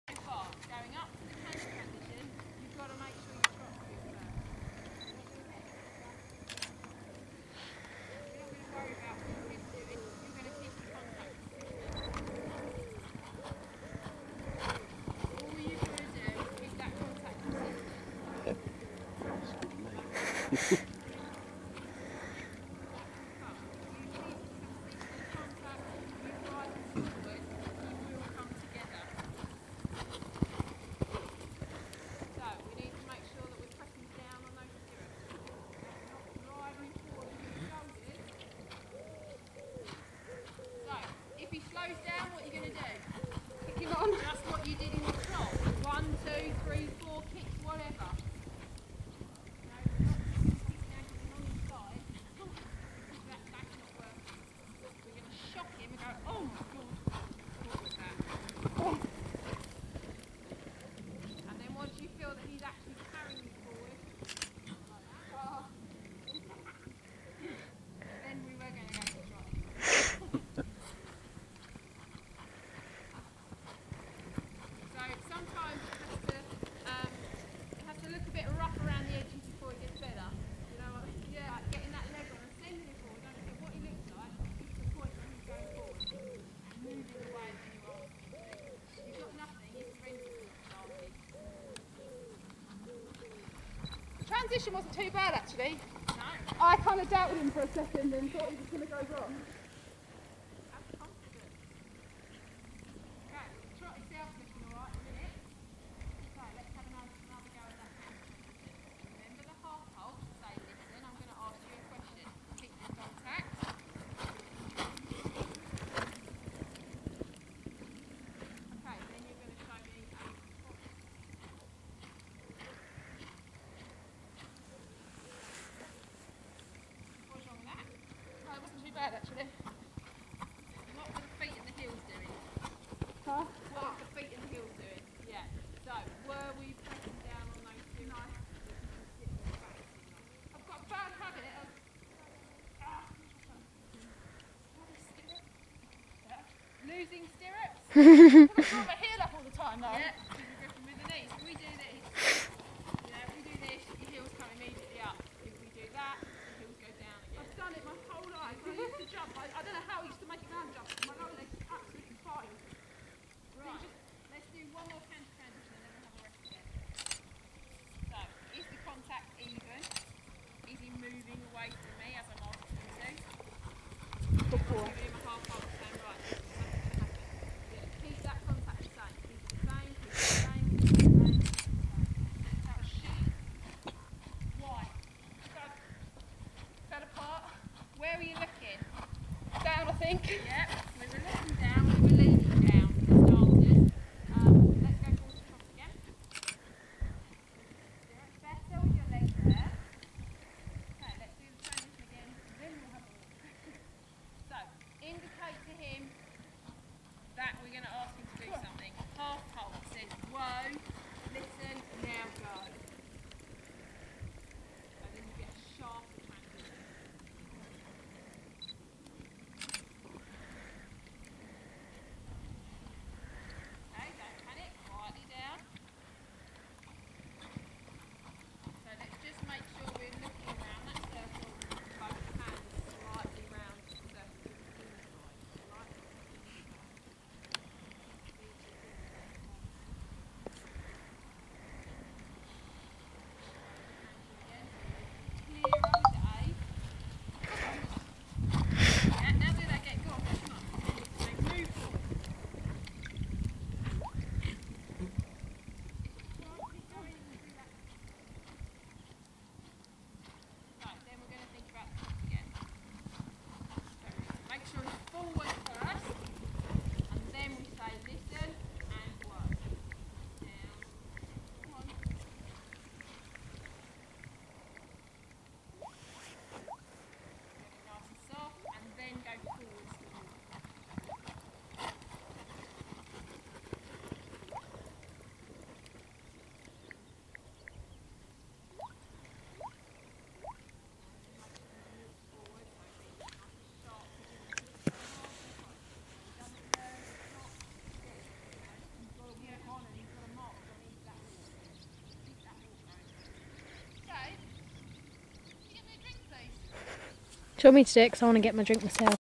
R rein canter vb lesson 20/8/13
Right rein canter work, also pretty awful but we'll get there